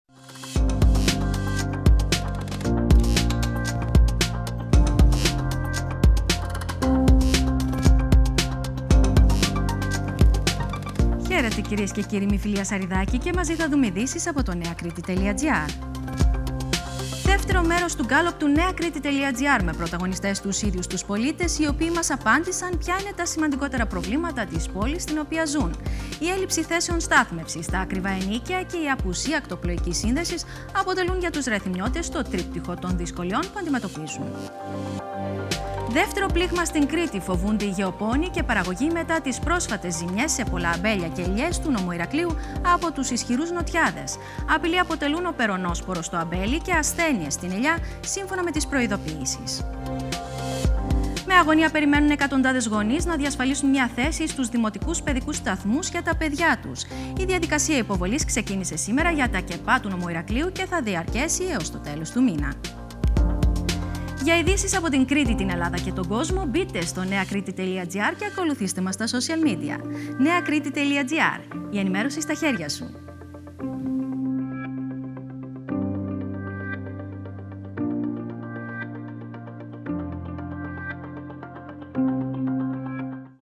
Εκλογές 2019: Οι Ρεθυμνιώτες μιλούν - Αυτά είναι τα προβλήματα στο Ρέθυμνο
Δεύτερο μέρος του γκάλοπ του Νeakriti.gr με πρωταγωνιστές τους ίδιους τους πολίτες οι οποίοι μας απάντησαν ποια είναι τα σημαντικότερα προβλήματα της πόλης στην οποία ζουν. Η έλλειψη θέσεων στάθμευσης, τα ακριβά ενοίκια και η απουσία ακτοπλοϊκής σύνδεσης αποτελούν για τους Ρεθυμνιώτες το τρίπτυχο των δυσκολιών που αντιμετωπίζουν.

Με τη Φιλία Σαριδάκη